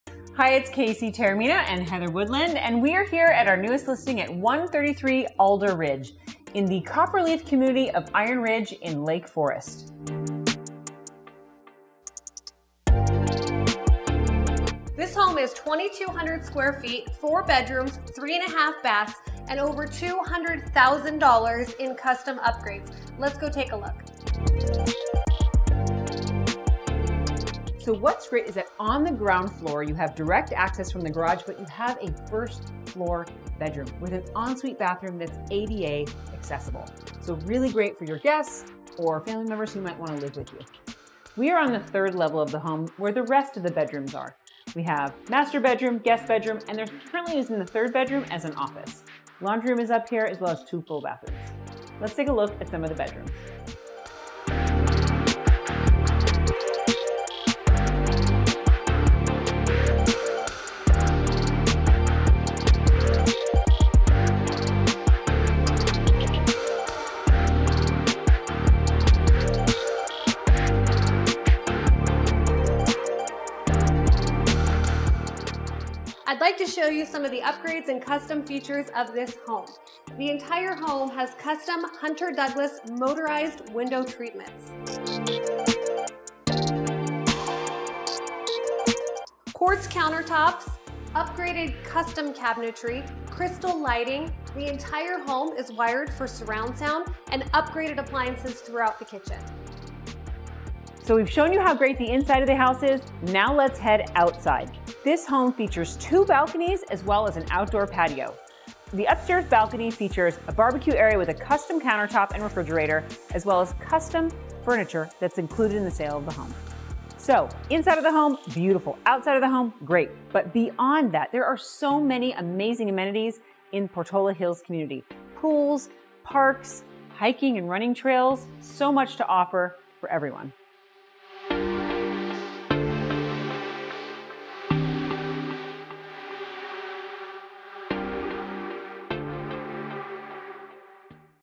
133 Alder Ridge, Lake Forest
Located in the beautiful Iron Ridge development in the Portola Hills area, this newly constructed 4 bedroom, 3.5 Bath home features over $200,000 in custom upgrades throughout. Luxury and comfort unite with upgrades such as custom countertops, cabinetry, light fixtures, and appliances in the chefs' kitchen as well as the whole home surround system, luxury vinyl flooring, and custom window shades including plantation shutters and electric Hunter Douglas shades. A first-floor guest bedroom suite includes a fully ADA-accessible bathroom and patio. The second level enjoys a wide-open floor plan, huge kitchen, and living room, as well as views from both of the two balcony decks with custom built-in barbeque and custom-made outdoor furniture. The 3rd level has 3 bedrooms plus the laundry room, with one of the bedrooms currently being used as an office with custom cabinetry. The Smart Home system will allow you to set the alarm system, open the garage, unlock the doors and even turn on the surround sound all with the touch of a button. Located within the private Iron Ridge community with access to hiking trails, and numerous association amenities, this is a wonderful place to call home.